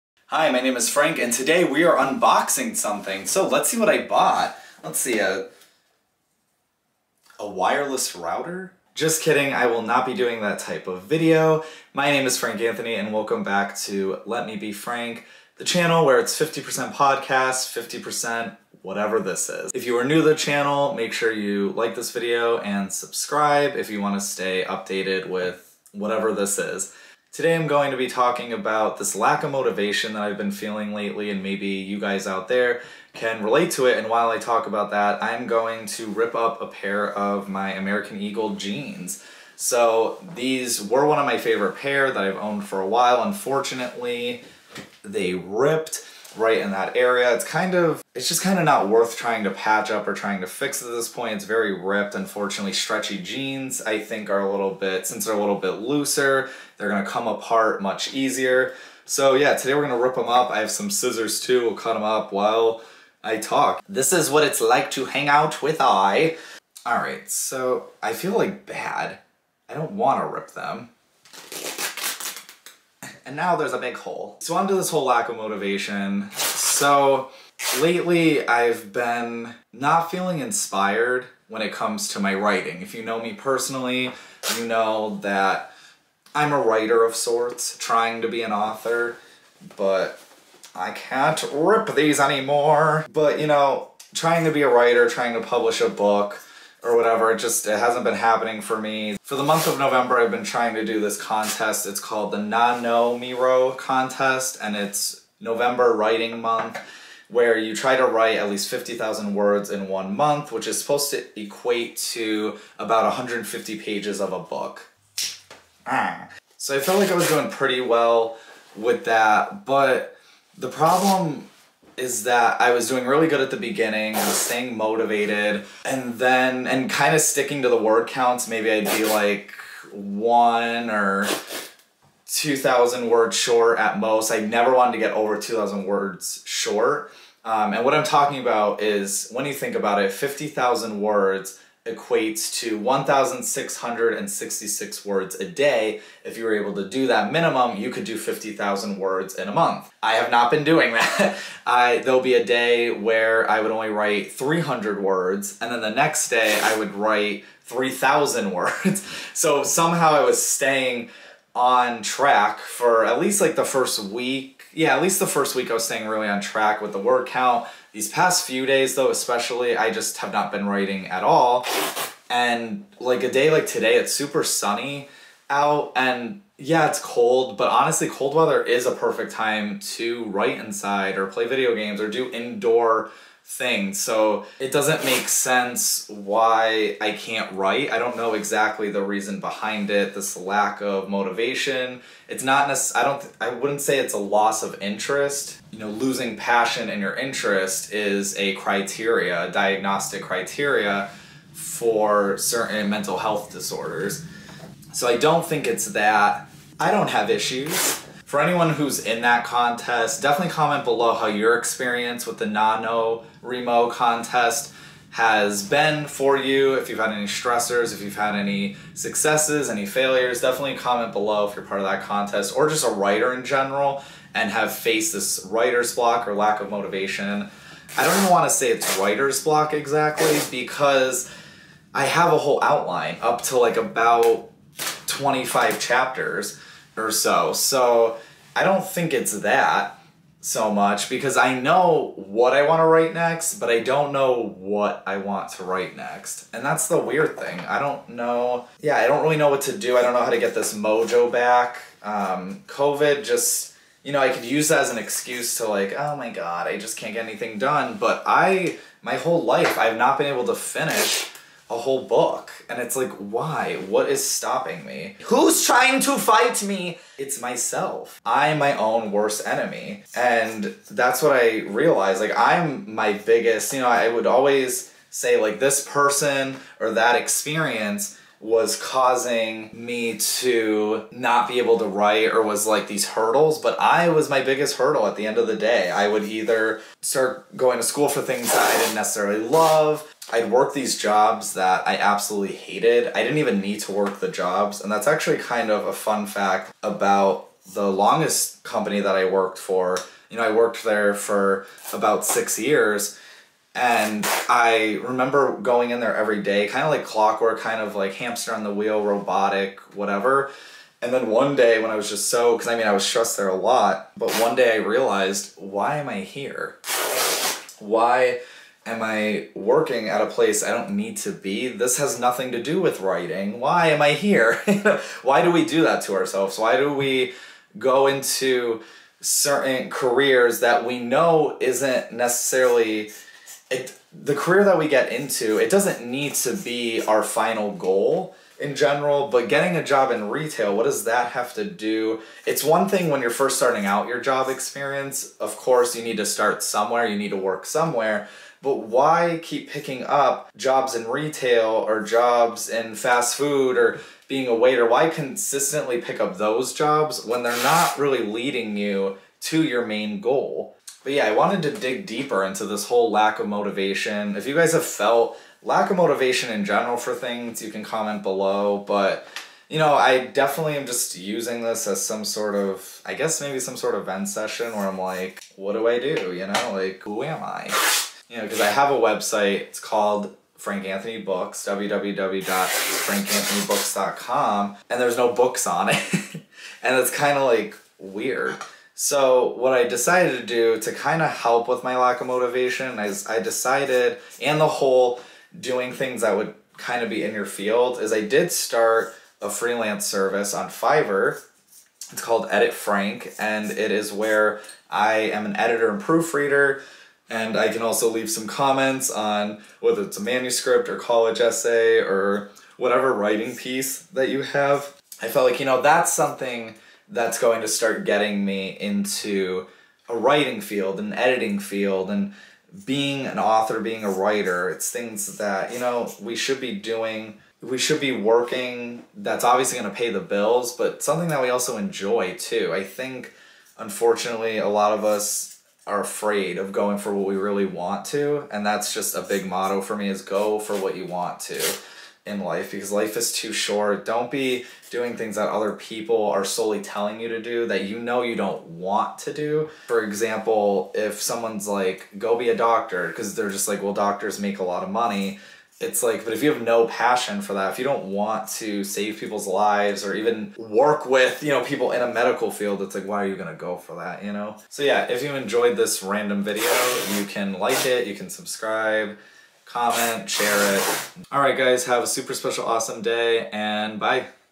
A Lack of Motivation (ft. Ripped Jeans)
Today I cut up a pair of ripped jeans while I try to figure out why I'm so useless.

Producers:
Monica Leigh